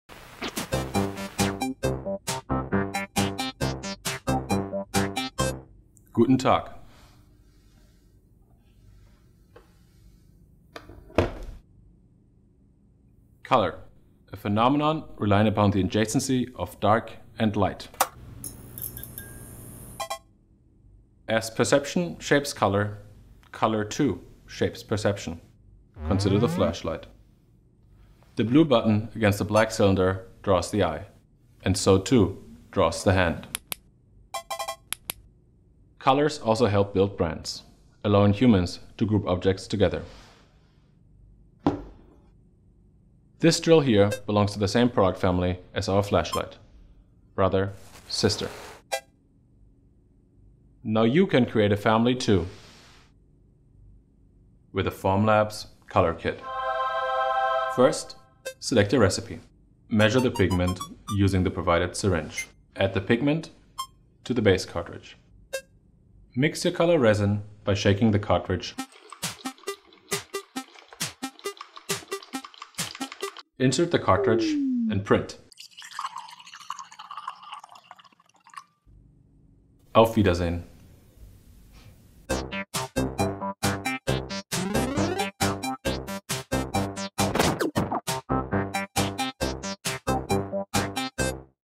3D Print in Color with Formlabs Color Kit
Formlabs is pleased to announce Color Kit, the first integrated color mixing solution for stereolithography 3D printing.

In this video, we will show you how to 3D print colored parts. Using Color Kit, anyone can mix their own Color Resin and create uniformly colored 3D prints with the high resolution and smooth surface finish SLA technology is known for.

For looks-like prototypes, concept models, and others parts where aesthetic is a top priority, Color Kit enables you to print many models in one consistent color, right off the printer, without the additional work of finishing and painting.

Choose Color Resin for matte, opaque parts straight off the printer, or paint another material when your needs are a bit more complex. Color Kit and painting techniques can be used together if your models would benefit from a colored base.